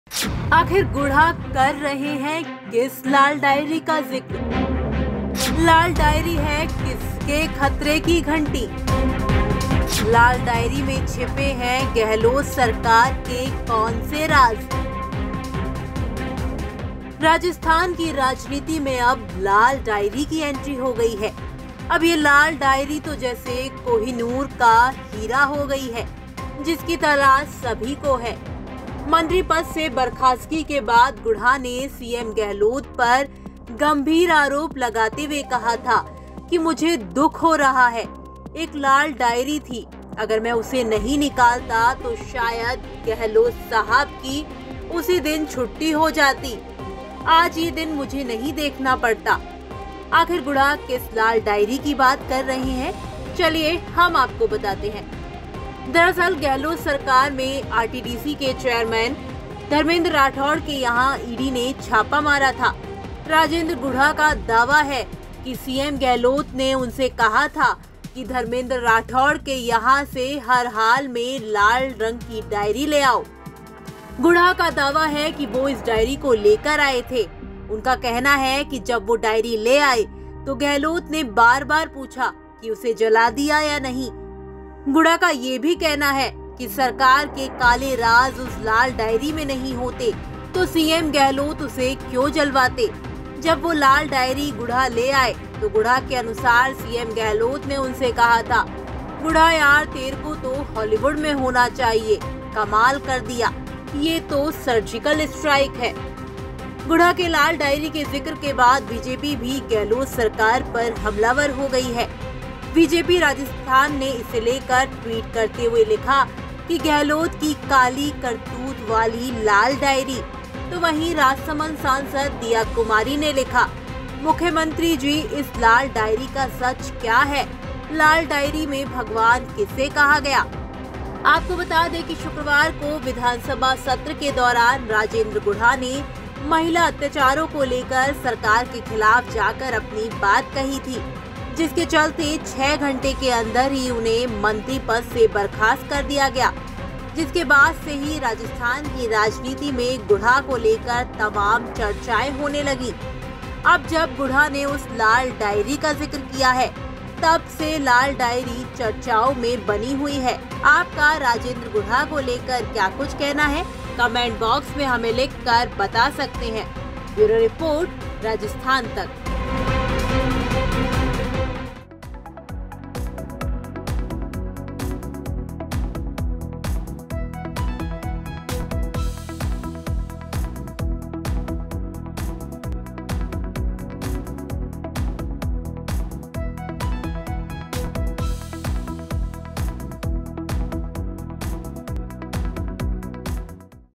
Rajendra Gudha की लाल डायरी है किसके खतरे की घंटी जिसकी बीजेपी को भी है तलाश?
राजस्थान की राजनीति में अब लाल डायरी की एंट्री हो गई है। अब ये लाल डायरी तो जैसे कोहिनूर का हीरा हो गई है।जिसकी तलाश सभी को है। मंत्री पद से बर्खास्तगी के बाद गुढ़ा ने सीएम गहलोत पर गंभीर आरोप लगाते हुए कहा था कि मुझे दुख हो रहा है, एक लाल डायरी थी, अगर मैं उसे नहीं निकालता था तो शायद गहलोत साहब की उसी दिन छुट्टी हो जाती. आज ये दिन मुझे नहीं देखना पड़ता. आखिर गुढ़ा किस लाल डायरी की बात कर रहे हैं, चलिए हम आपको बताते हैं।